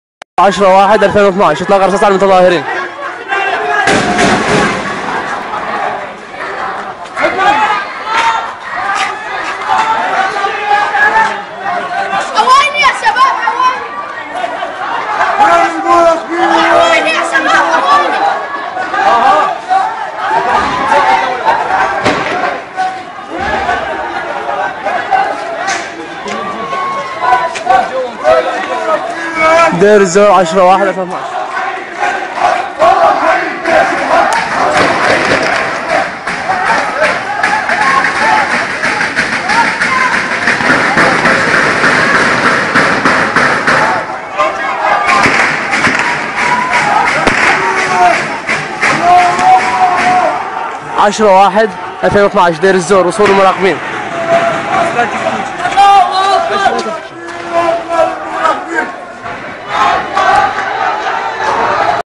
Heavy Fire at Protesters by Security Forces, Deir Ezzor City, 10-Jan-12
مجلس ثوار ديرالزور أحرار ديرالزور يطالبون بتدخل الجيش الحر لحمايتهم 10 1